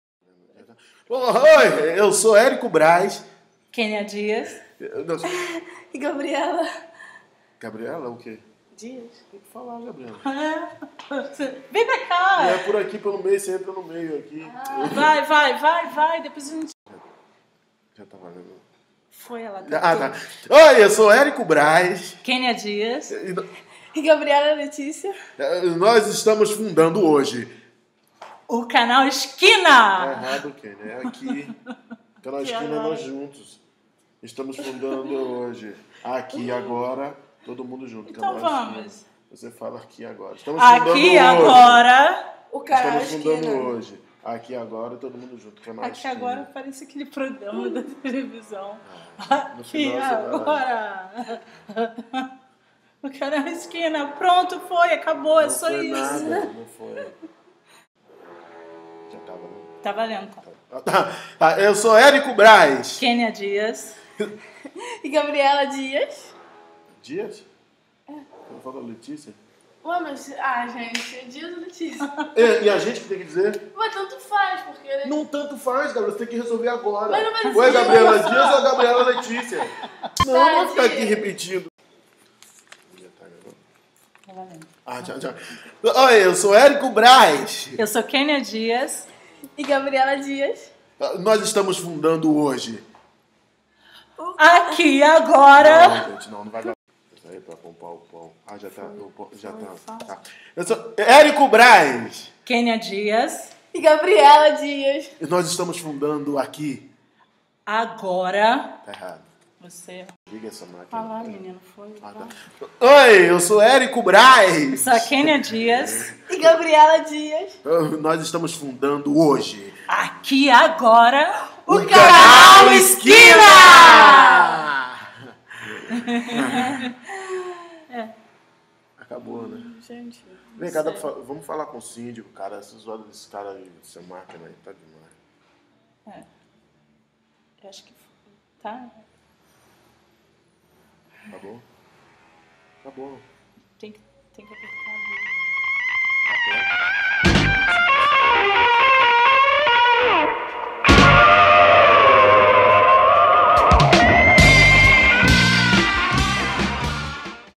Tá bom pra você? LANÇAMENTO.
Esse video é um registro de Fundação do Canal Esquina por Érico Brás,Kenia Dias e Gabriela Dias.
Uma realização da Kbra produções.